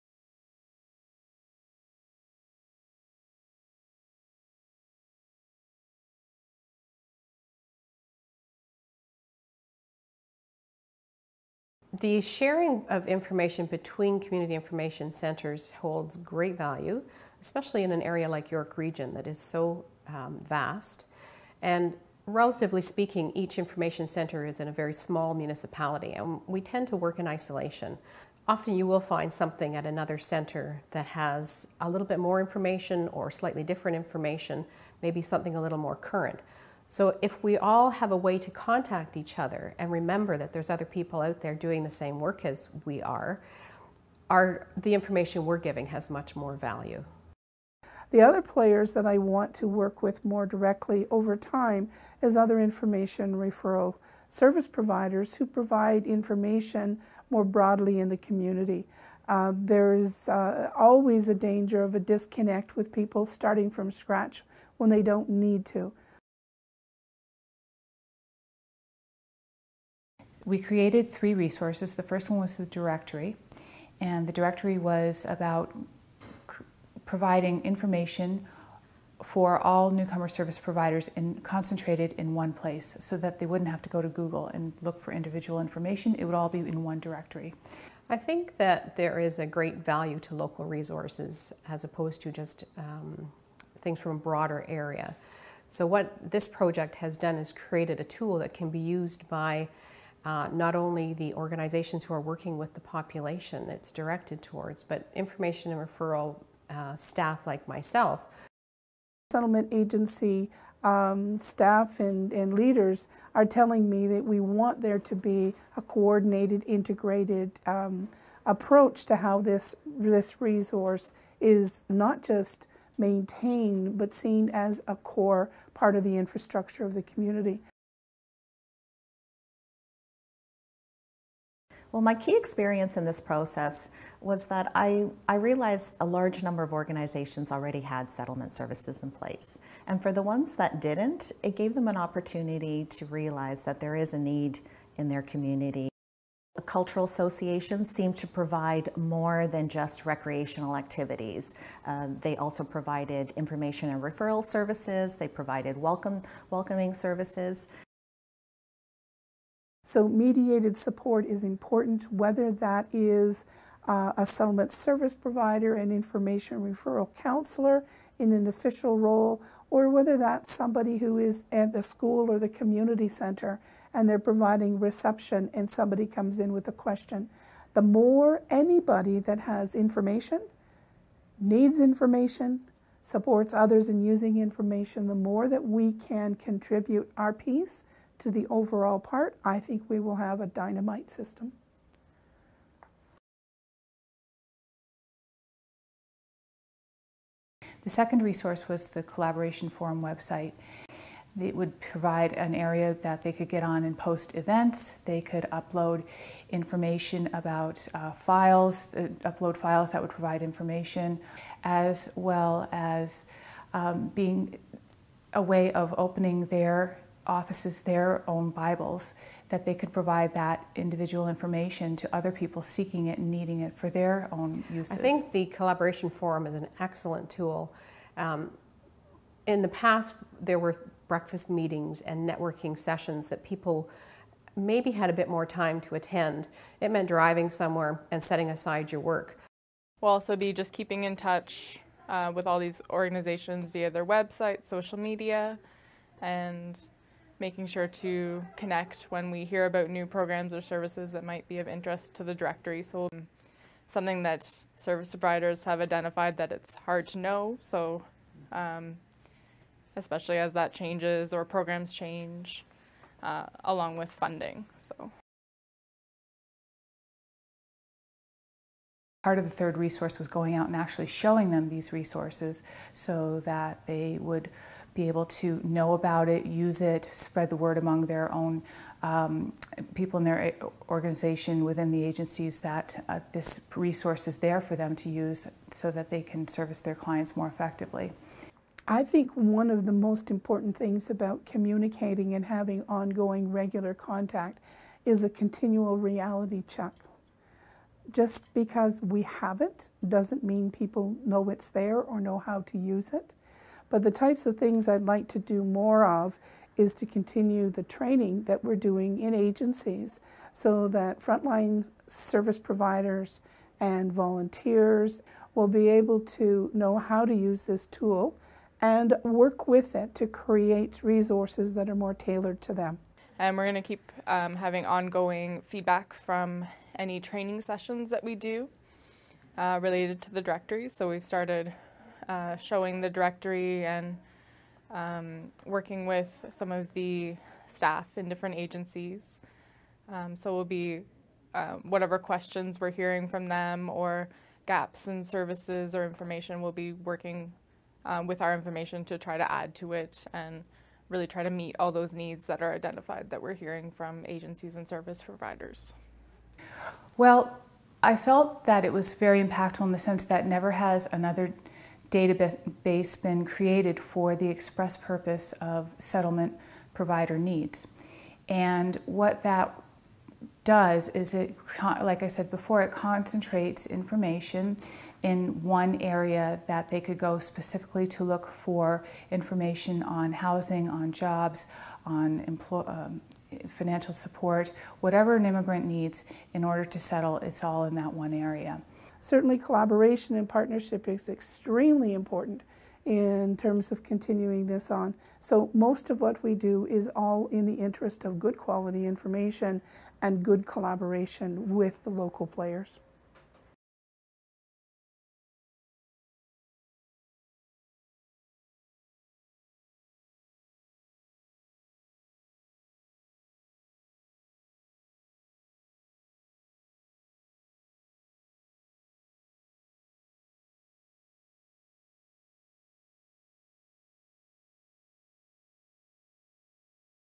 Community Information Management - BLISON 2014
Staff from the two project pilot communities, York Region and Waterloo Region, share what their work is like in the current local information and referral systems.